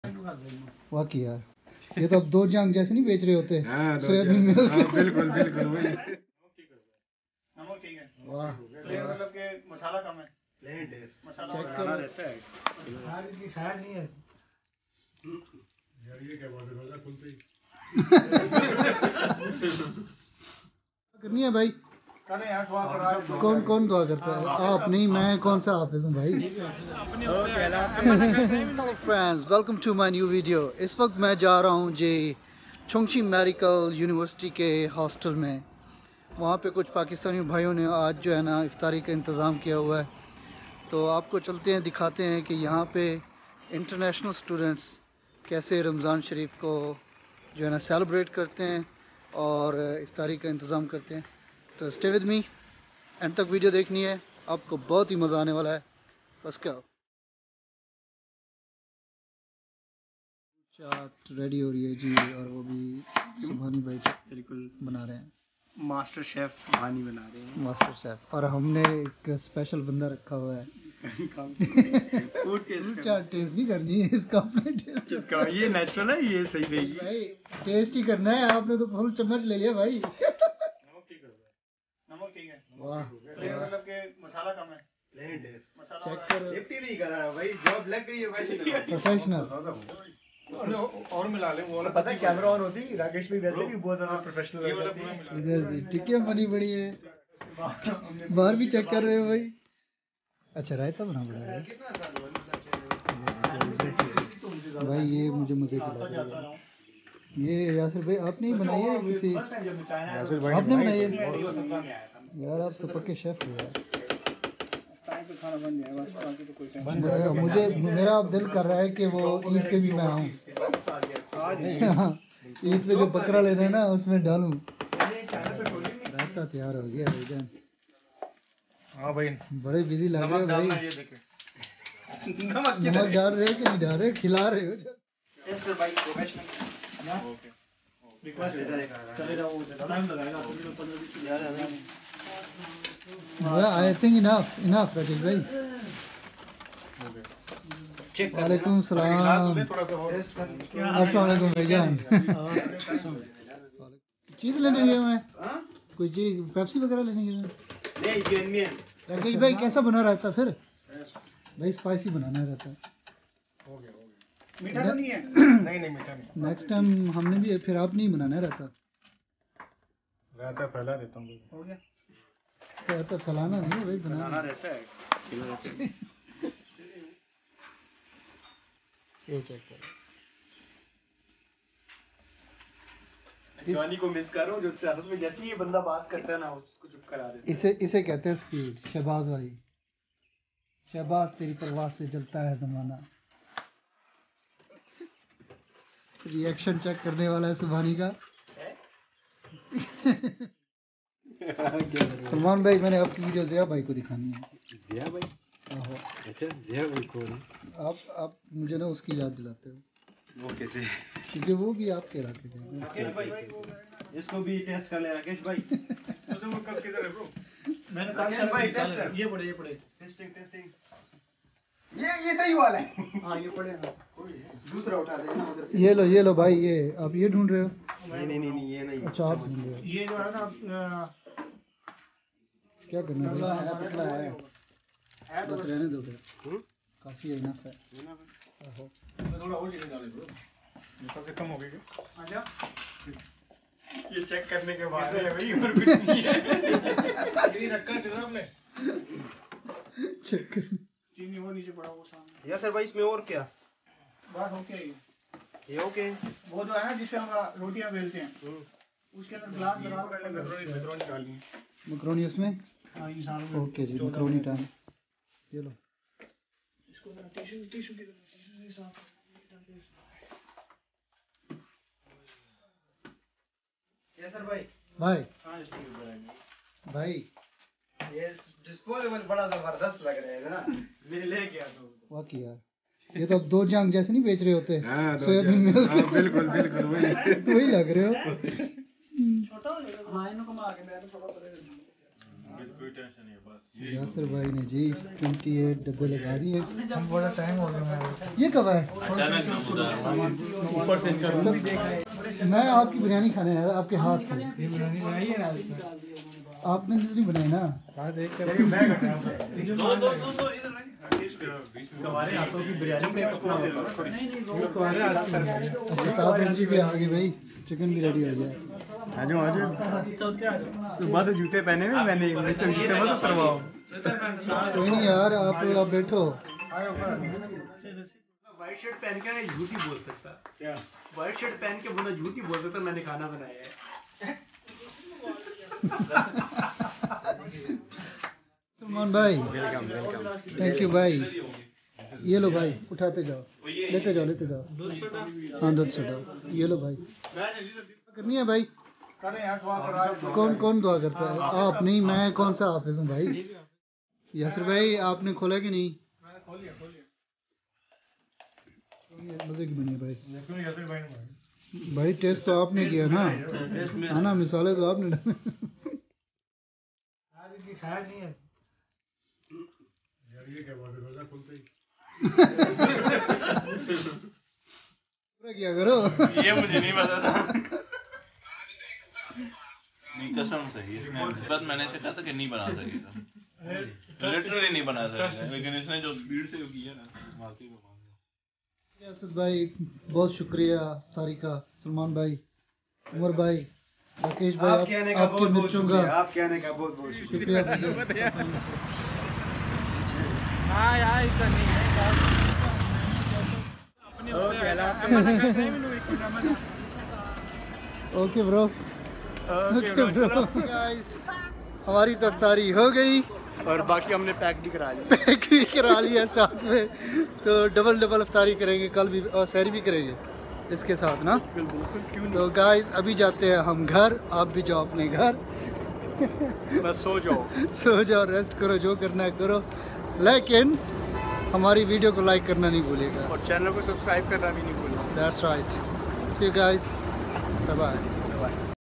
iftar party in university hostel | CHINA | iftari with Pakistani students | CQMU | Ramdan 2023
some friends living in chongqing medical university, planned an iftar party. He cooked the delicious food and we had quality time after a long time.